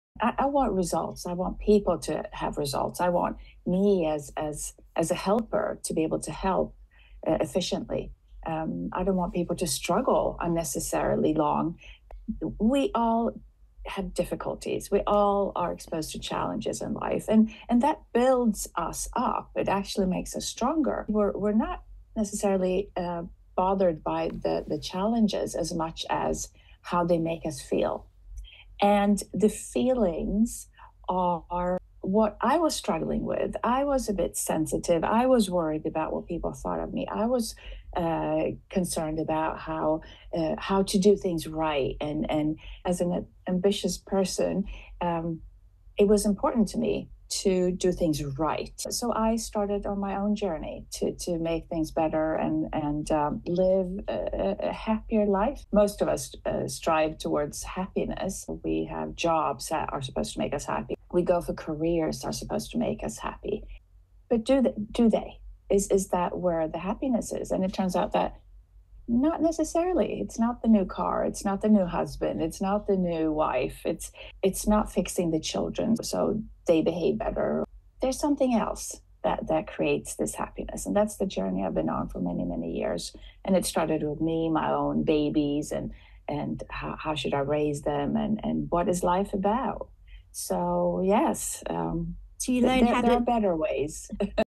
So who is Joanna Armstrong? This will reveal her passion and motivation in life.
Mainly, I focus on efficient solutions to eliminate troubling feelings and emotions, such as stress, worry, fear, sleep difficulty, and many more.

WELCOME!